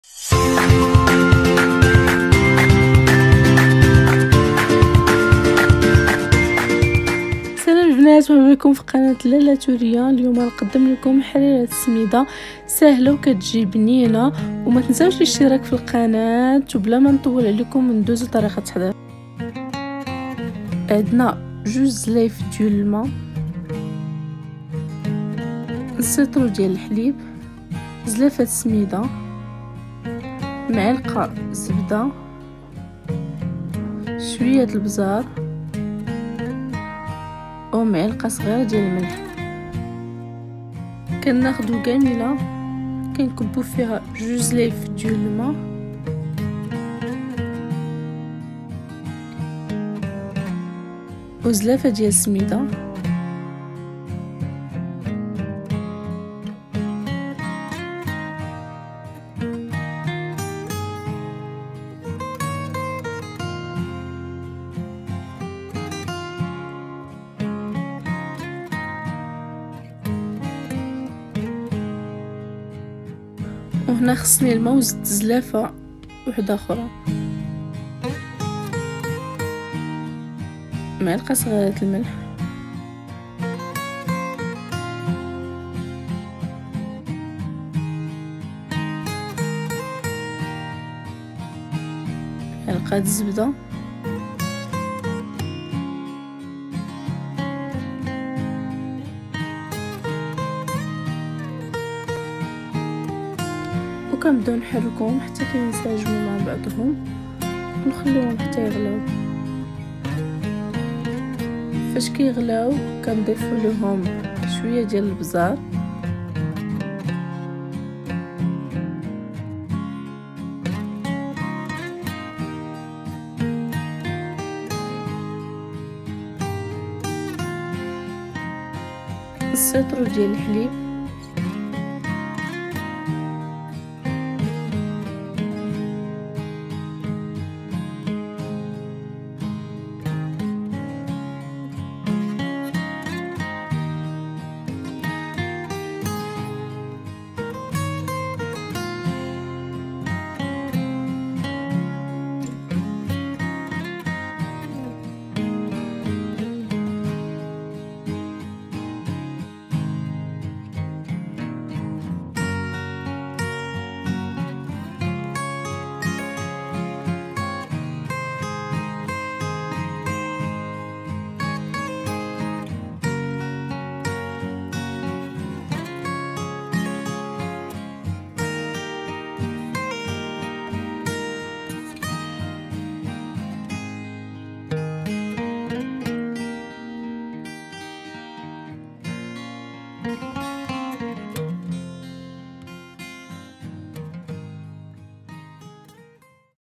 طريقة تحضير حريرة السميد المغربية 🇲🇦 بالطريقة الأصلية الناجحة🍲
لالة ثورية تقدم لكم وجبة سهلة وسريعة تحضير حريرة السميد المغربية بطريقة جديدة رائعة